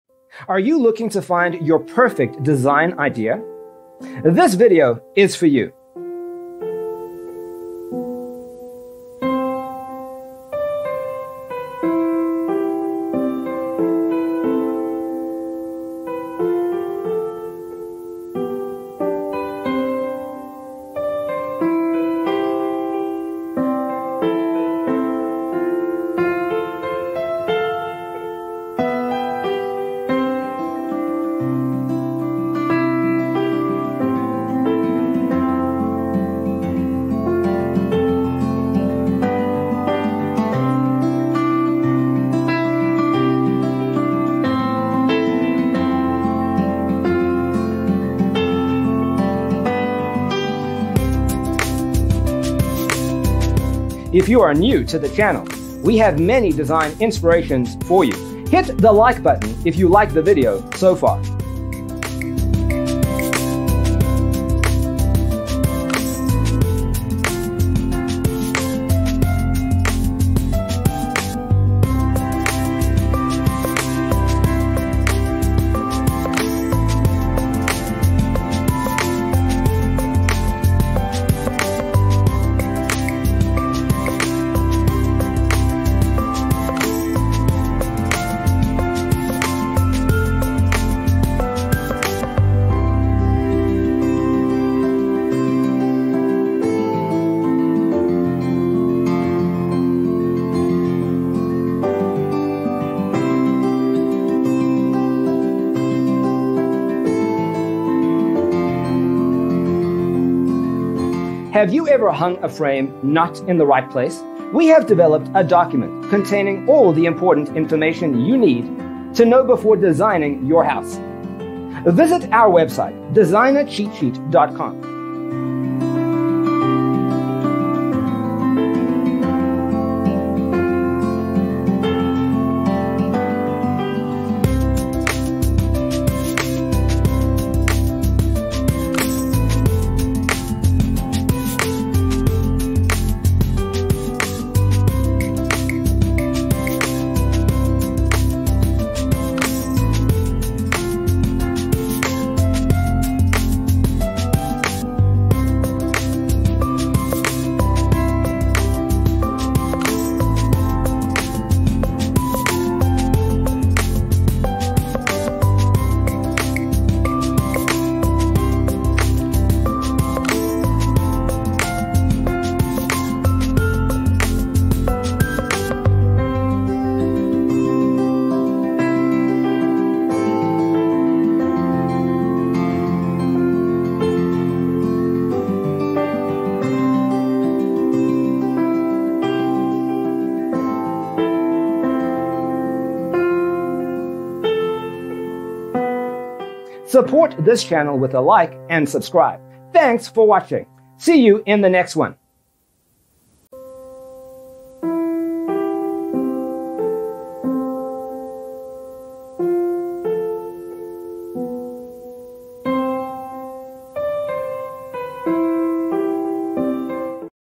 75 Beautiful Blue Living Room With A Hanging Fireplace Design Ideas #1345 💙
Browse blue living room decorating ideas and furniture layouts. Discover design inspiration from a variety of blue living rooms, including color, decor and storage options.

0:00 - Intro
0:00:12 -  #1
0:00:17 -  #2
0:00:22 -  #3
0:00:27 -  #4
0:00:32 -  #5
0:00:37 -  #6
0:00:42 -  #7
0:00:47 -  #8
0:00:52 -  #9
0:00:57 -  #10
0:01:02 -  #11
0:01:08 -  #12
0:01:14 -  #13
0:01:20 -  #14
0:01:26 -  #15
0:01:32 -  #16
0:01:38 -  #17
0:01:44 -  #18
0:01:50 -  #19
0:01:56 -  #20
0:02:02 -  #21
0:02:09 -  #22
0:02:16 -  #23
0:02:23 -  #24
0:02:30 -  #25
0:02:37 -  #26
0:02:44 -  #27
0:02:51 -  #28
0:02:58 -  #29
0:03:05 -  #30
0:03:12 -  #31
0:03:19 -  #32